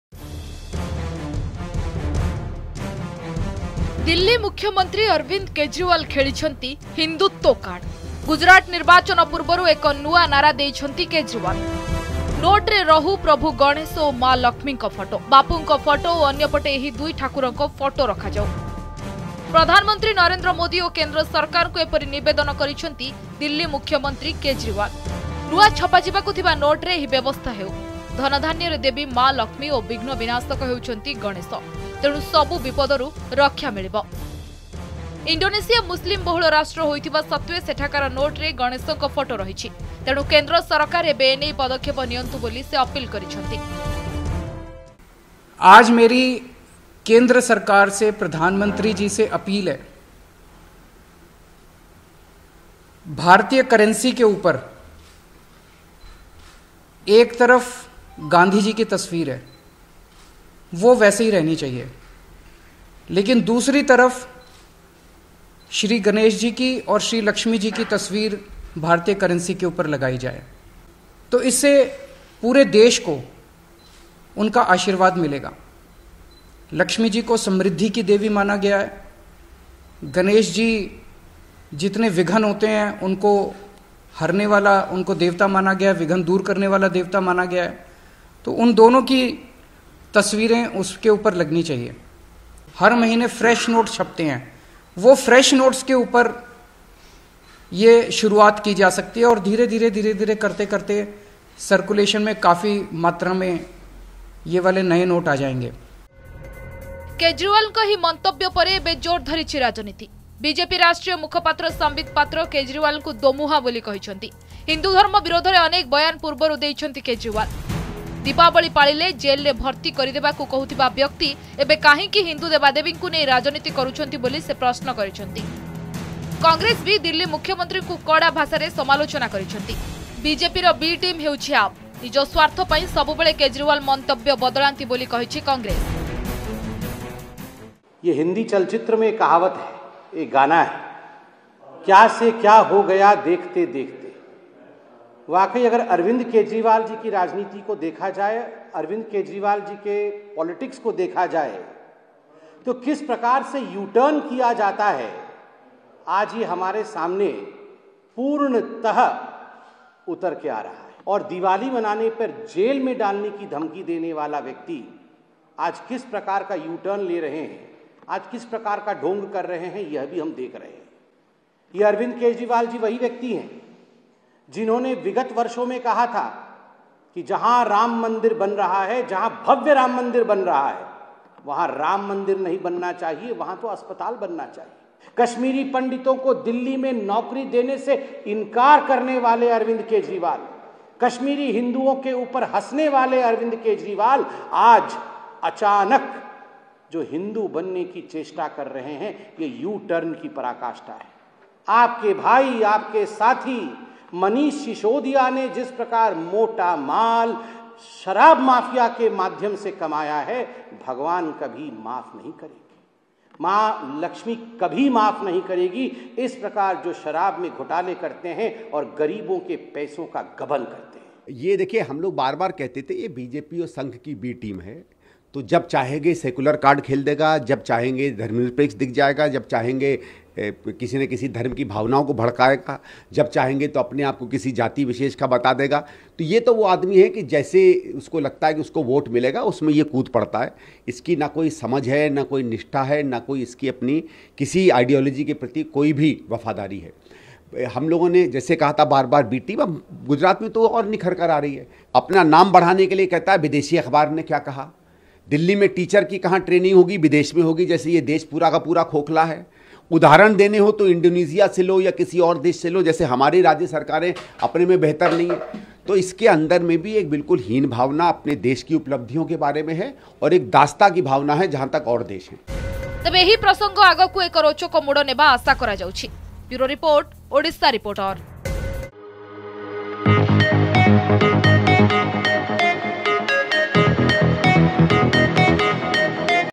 କେଜ୍ରିଓ୍ୱାଲଙ୍କ ହିନ୍ଦୁତ୍ୱ କାର୍ଡ | Odisha Reporter
ଦିଲ୍ଲୀ ମୁଖ୍ୟମନ୍ତ୍ରୀ ଅରବିନ୍ଦ କେଜ୍ରିଓ୍ୱାଲ ଖେଳିଛନ୍ତି ହିନ୍ଦୁତ୍ୱ କାର୍ଡ। ଗୁଜରାଟ ନିର୍ବାଚନ ପୂର୍ବରୁ ଏକ ନୂଆ ନାରା ଦେଇଛନ୍ତି କେଜରିଓ୍ୱାଲ। ନୋଟ୍ରେ ରହୁ ପ୍ରଭୁ ଗଣେଶ ଓ ମା ଲକ୍ଷ୍ମୀଙ୍କ ଫଟୋ । ବାପୁଙ୍କ ଫଟୋ ଓ ଅନ୍ୟପଟେ ଏହି ଦୁଇ ଠାକୁରଙ୍କ ଫଟୋ ରଖାଯାଉ । ପ୍ରଧାନମନ୍ତ୍ରୀ ନରେନ୍ଦ୍ର ମୋଦି ଓ କେନ୍ଦ୍ର ସରକାରଙ୍କୁ ଏପରି ନିବେଦନ କରିଛନ୍ତି ଦିଲ୍ଲୀ ମୁଖ୍ୟମନ୍ତ୍ରୀ କେଜରୀଓ୍ବାଲ୍‌ ।